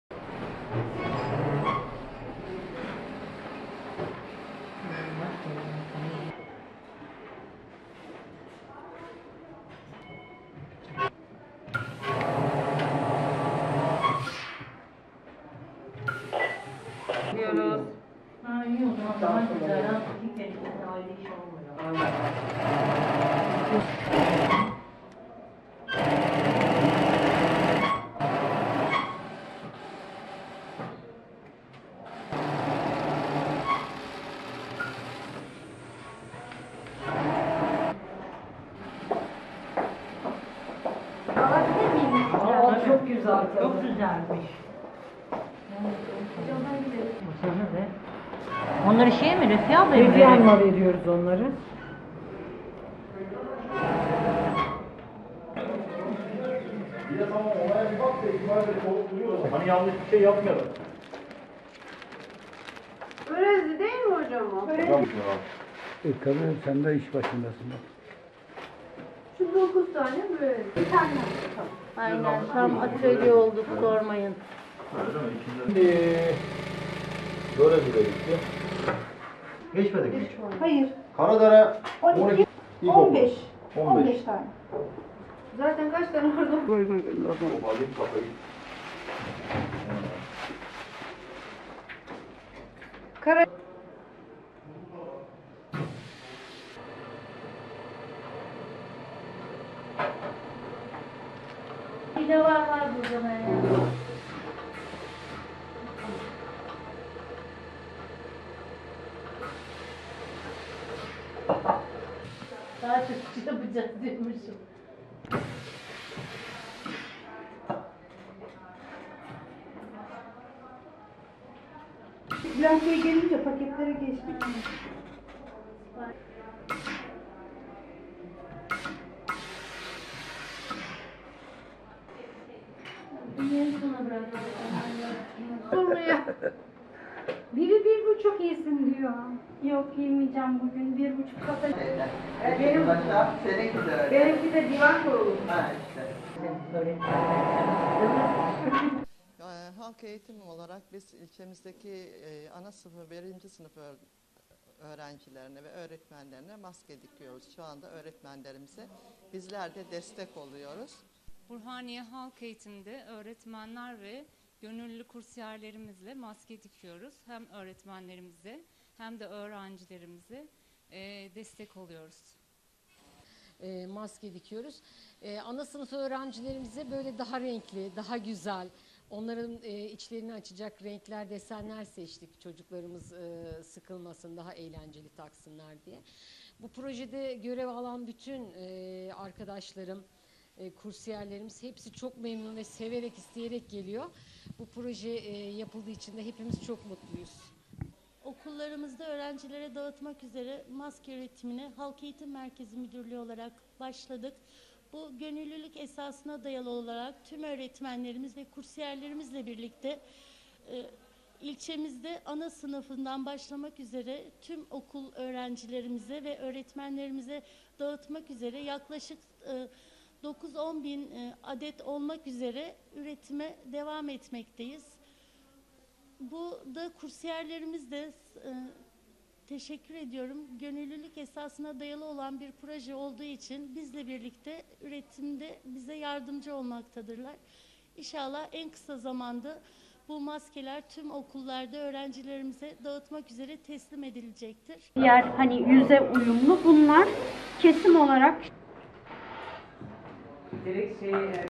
HEM kursiyerleri imeceyle maske dikiyor
Balıkesir'in Burhaniye ilçesinde, Halk Eğitimi Müdürlüğünde görevli 6 el sanatları öğretmeni ile 15 kursiyer kadın maske üretiyor.